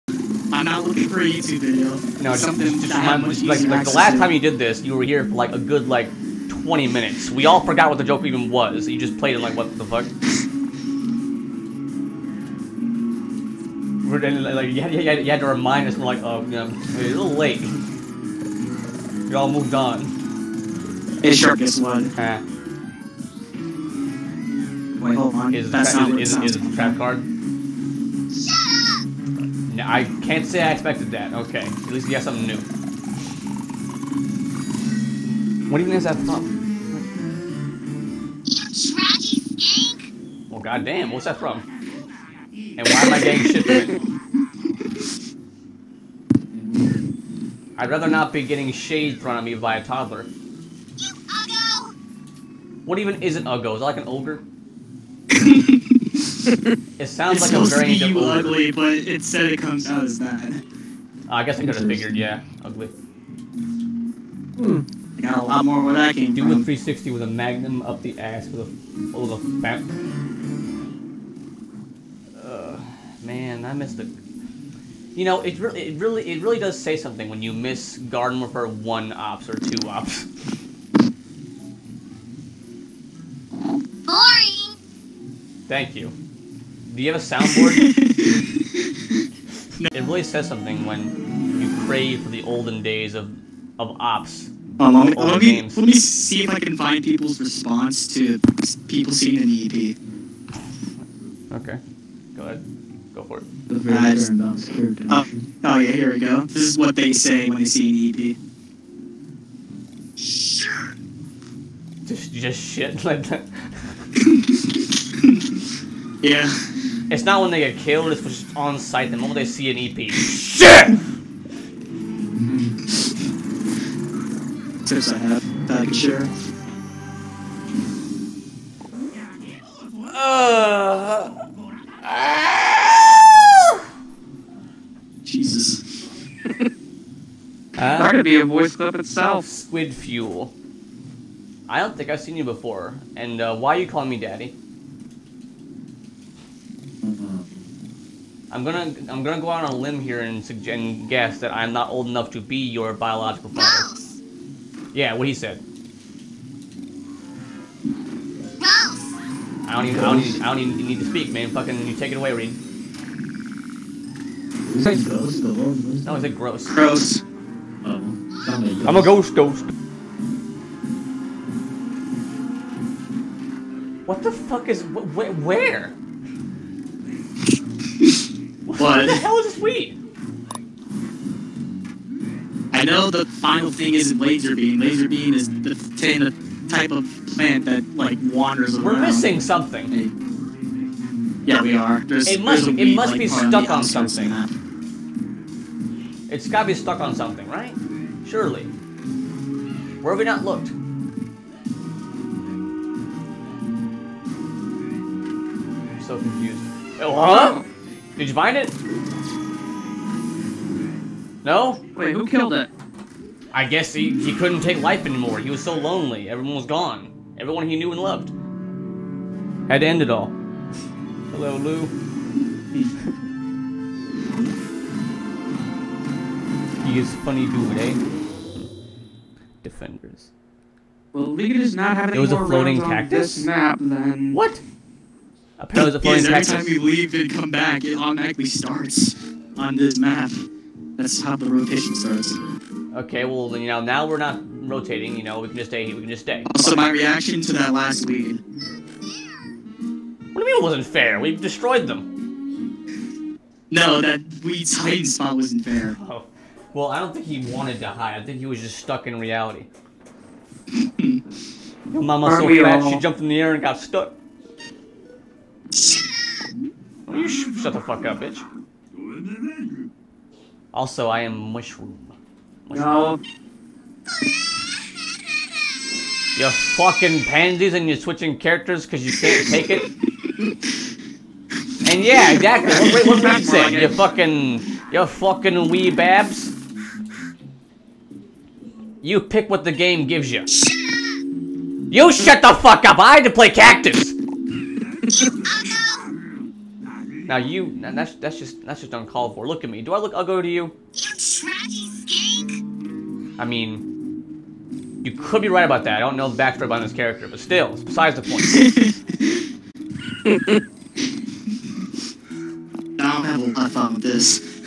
Reid's Bootleg Soundboard
Twas a normal day of BfN gaming with a hint of Reid playing a different game&#39;s sound clips through his phone.